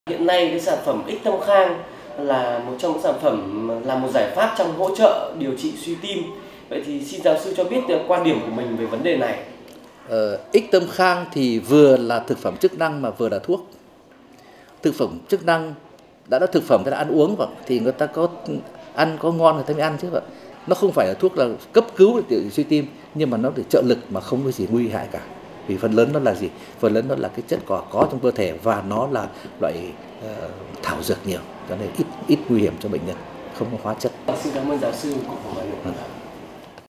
Ích Tâm Khang - Hỗ trợ điều trị bệnh tim mạch hiệu quả
Hiệu quả của Ích Tâm Khang trong điều trị các bệnh tim mạch đã được chứng minh trên lâm sàng và công bố trên tạp chí quốc tế năm 2014. Dưới đây là quan điểm của Gs. Phạm Gia Khải - Nguyên chủ tịch hội Tim mạch VN về Ích Tâm Khang.
Theo Gs, Ích Tâm Khang vừa là thực phẩm chức năng vừa là thuốc, mà không gây hại gì cho người dùng
Nếu bạn có bất kỳ boăn khoăn nào cần tư vấn về bệnh hẹp/hở van tim cũng như các bệnh tim mạch khác, bạn có thể gọi tới số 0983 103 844 để được tư vấn